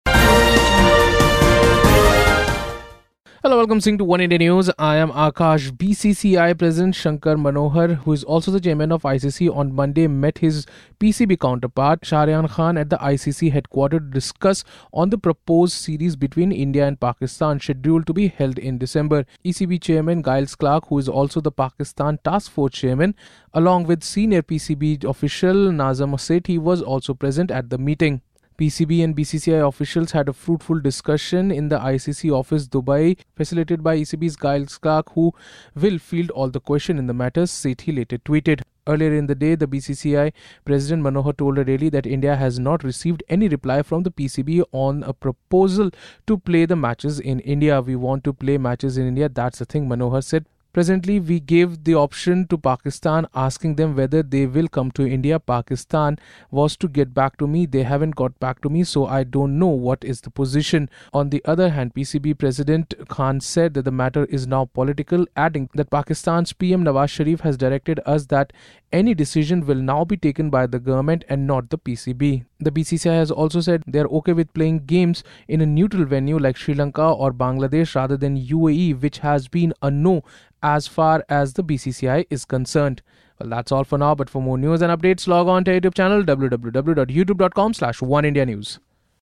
BCCI and PCB chiefs meet in Dubai, discuss Indo-Pak Series
BCCI president Shashank Manohar, on Monday met his PCB counterpart Shaharyar Khan at the ICC headquarters to discuss on the proposed series between India and Pakistan scheduled to be held in December. Earlier in the day, Manohar told a daily that India have not received a reply from the PCB on a proposal to play the matches in India.